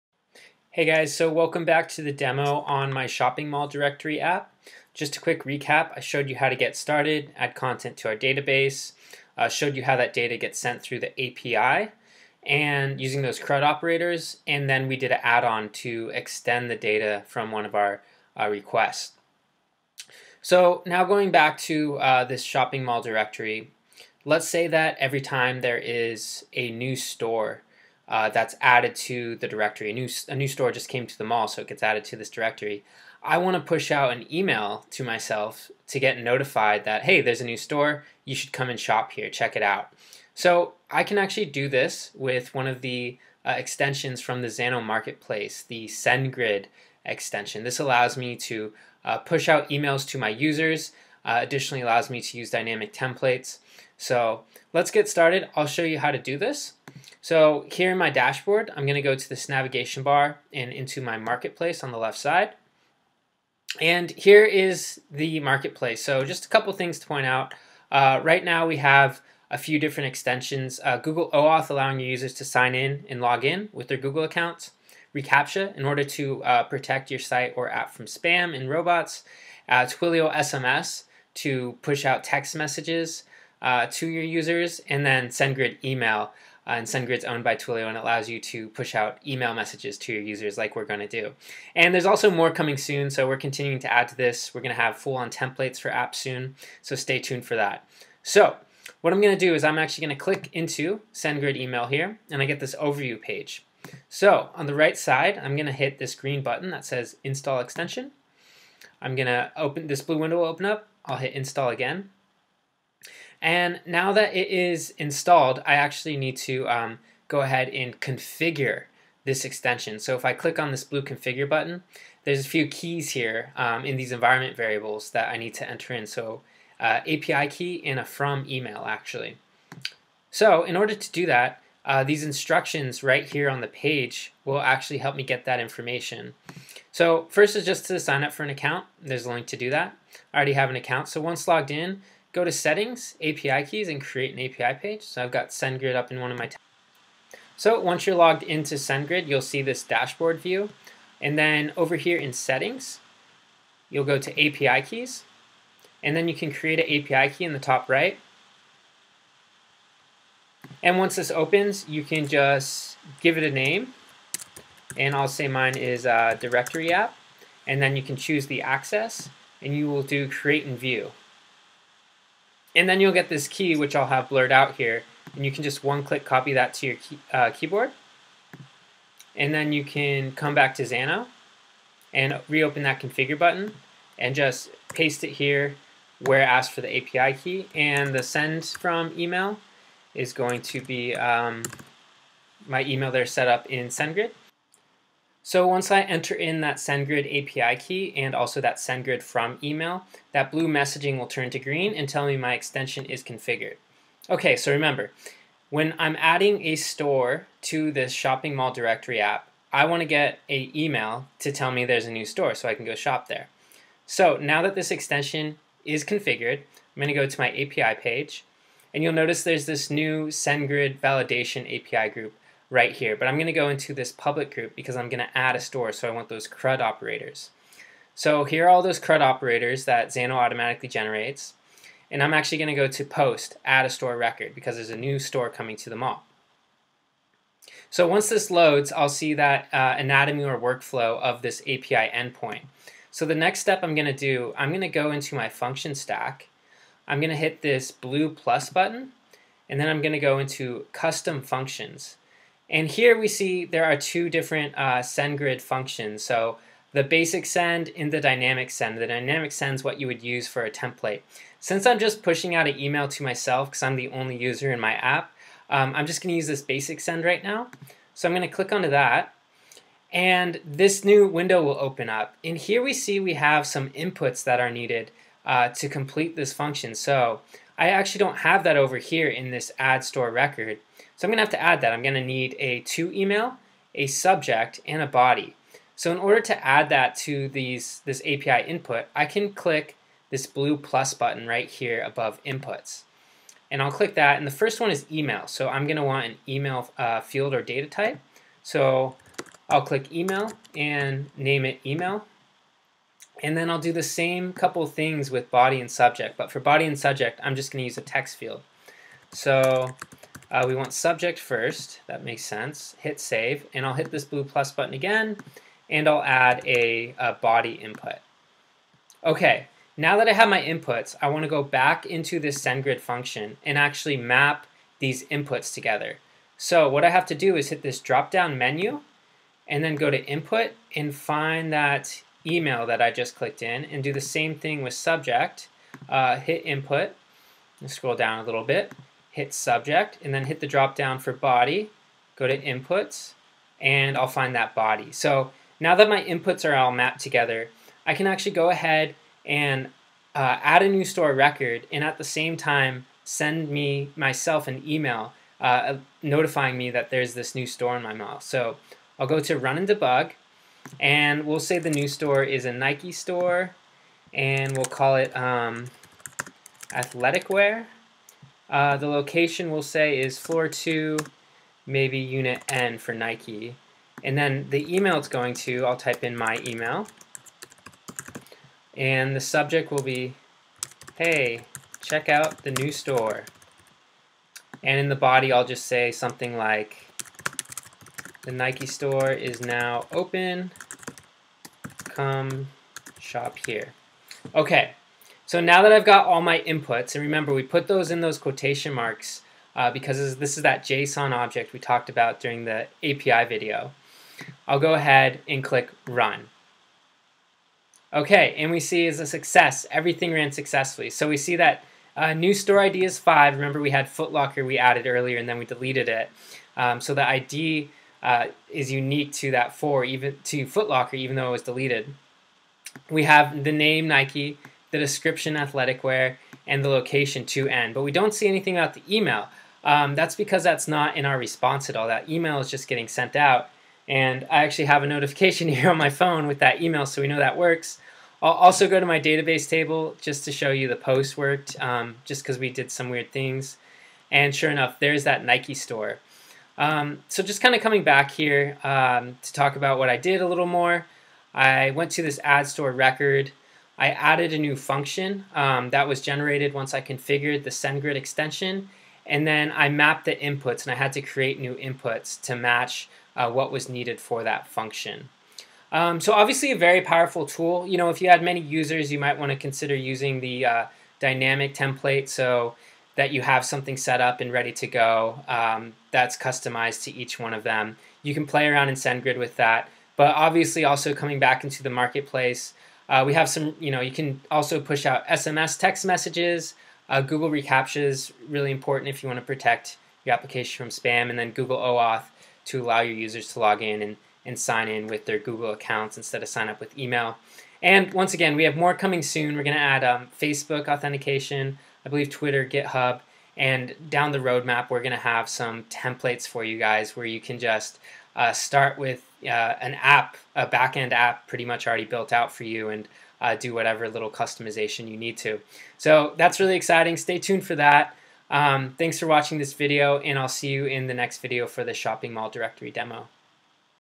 Demo: Shopping Mall Directory #4 - Sendgrid Marketplace Extension
In this part of the demo you will learn how to install and configure the Sendgrid Extension from the Xano Marketplace. Then, you will see how to implement it into an API endpoint to push a notification email when a new store is added to the directory app!